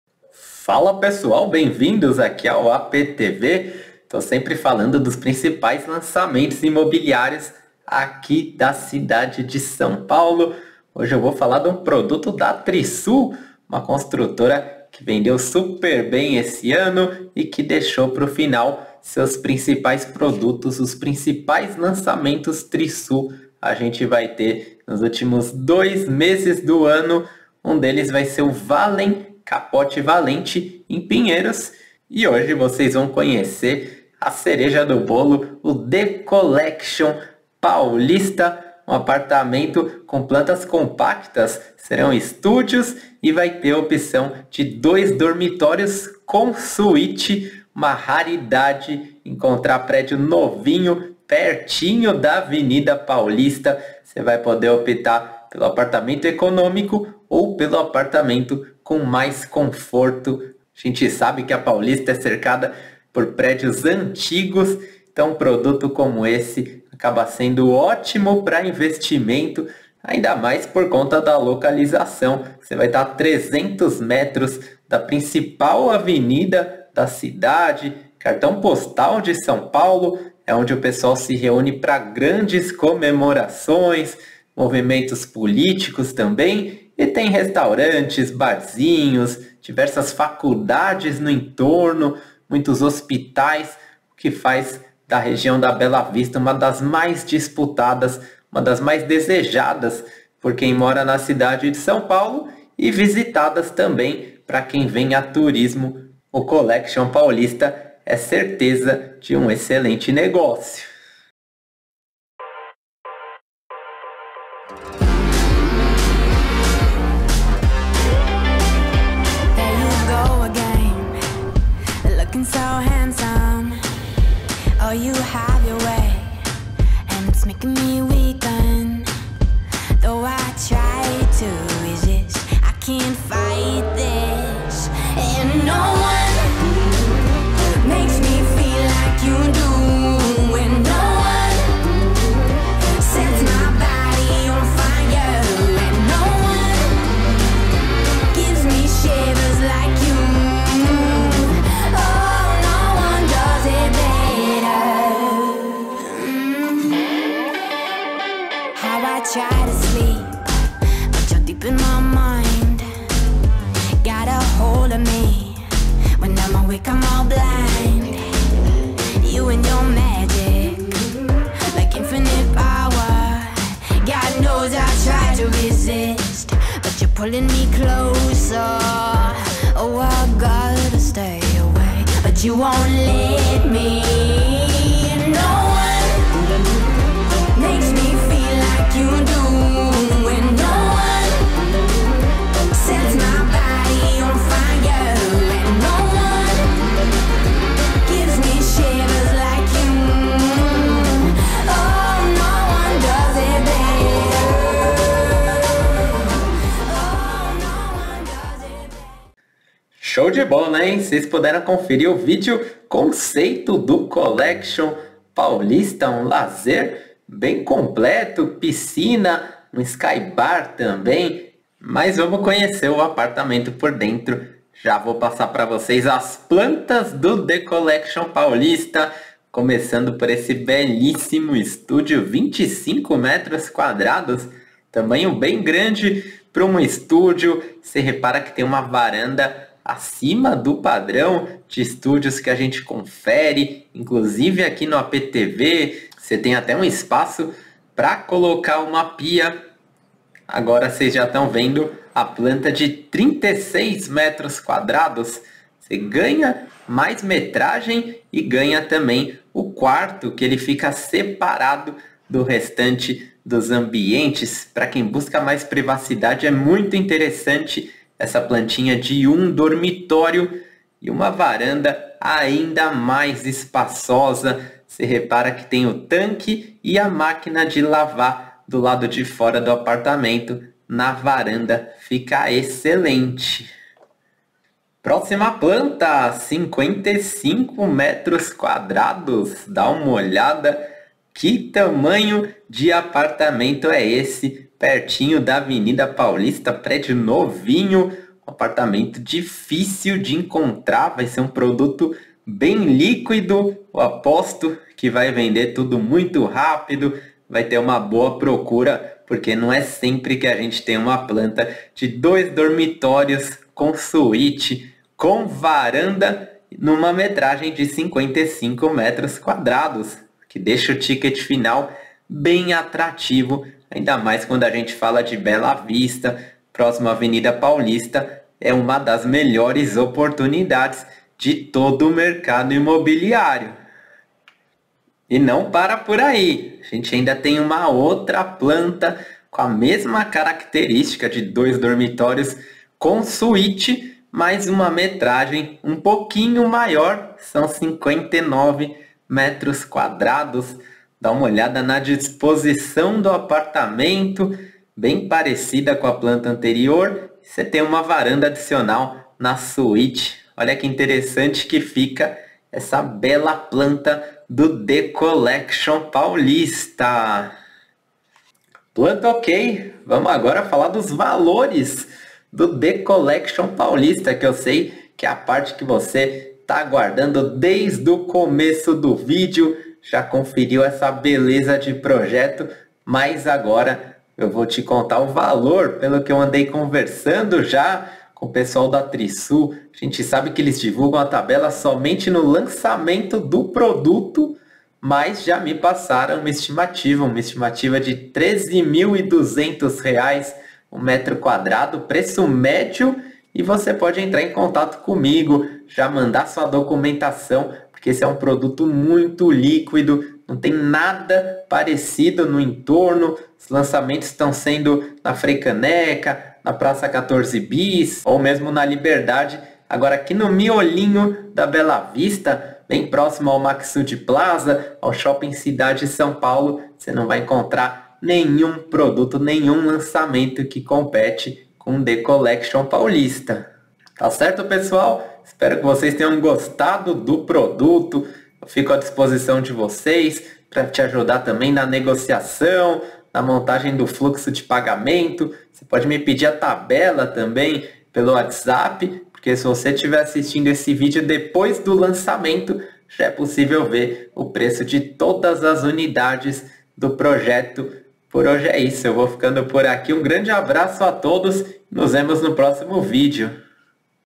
The Collection Paulista | Aptos de 25 a 59m² na Bela Vista | Lançamento Trisul a 350m da Paulista
Fala pessoal, está no ar mais um vídeo aqui no Apê TV. O tema da redação de hoje é o The Collection Paulista, novo apartamento da Trisul na Bela Vista, a 300 metros da principal avenida de São Paulo e 5 minutinhos caminhando da estação brigadeiro do Metrô.

Apresentação feita, chega a hora do segundo parágrafo, aquele que eu conto sobre as plantas do apartamento. No The Collection Paulista são diversas opções para todos os gostos e bolsos. Você poderá optar por apartamentos compactos, do tipo studio ou 1 dormitórios, com plantas de 25m² e 35m² respectivamente, ambas com varanda. Caso tenha uma família maior, eu indico as plantas de 55 e 59m², com 2 dormitórios, suíte e vaga de garagem.

Já imaginou que onda você vai tirar morando pertinho da Paulista e podendo frequentar os bares, restaurantes, cinemas, shoppings e tudo que o entorno da Av. Paulista pode te oferecer?

Então trate logo de garantir o seu apartamento! Não é preciso ser nenhum gênio da matemática para calcular que essa é a melhor oportunidade de investimento da sua vida, e muito menos ser o bam bam bam nota 1000 da redação do Enem. Basta saber assinar o seu próprio nome que o apartamento é seu.

Se preferir, você pode enviar uma mensagem pelo WhatsApp clicando no link abaixo!

00:00 Endereço do The Collection Paulista
01:51 Áreas de Lazer do The Collection Paulista
03:30 Plantas do The Collection Paulista
06:08 Estimativa de Preço do The Collection Paulista